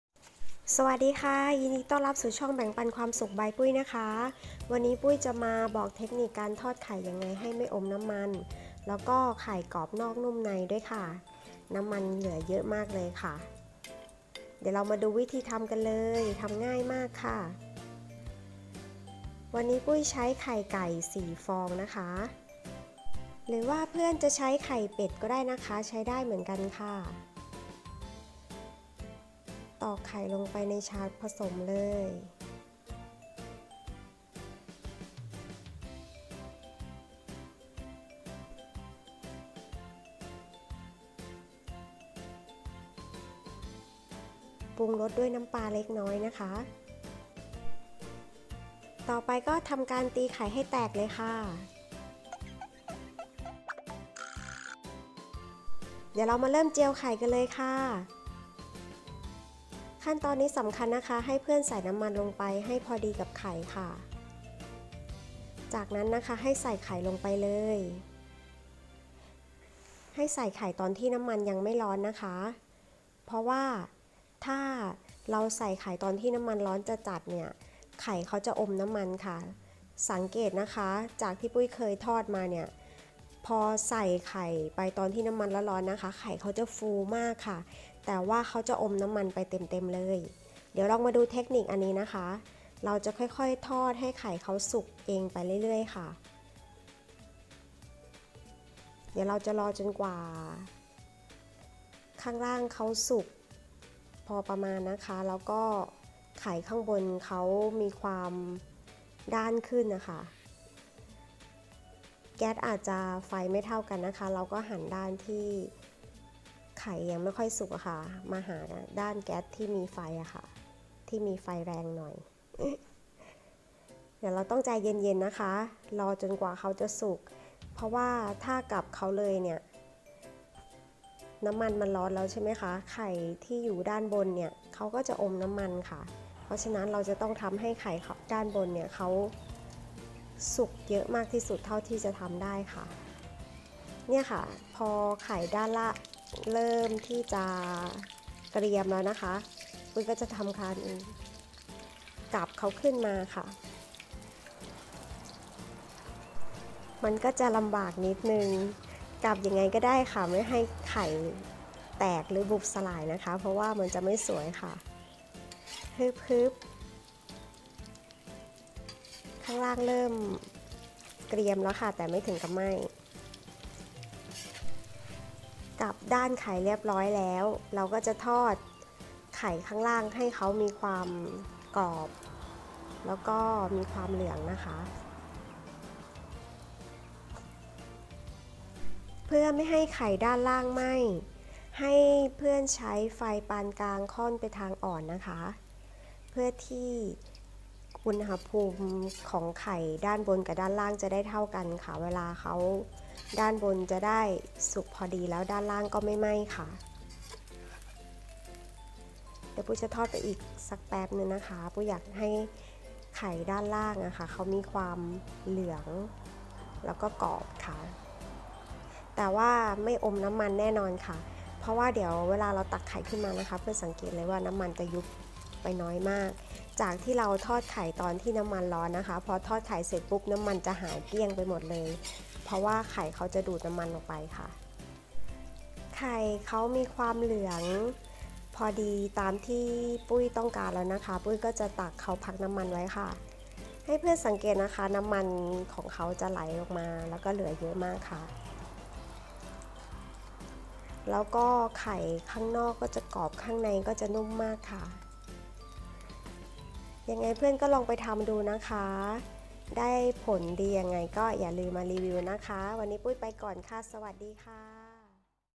วิธีทอดไข่เจียวให้กรอบนอกนุ่มในไม่อมน้ำมัน Thai omelet repice
เคล็ดลับทอดไข่ให้กรอบนอกนุ่มในไม่อมน้ำมันอร่อยมากๆ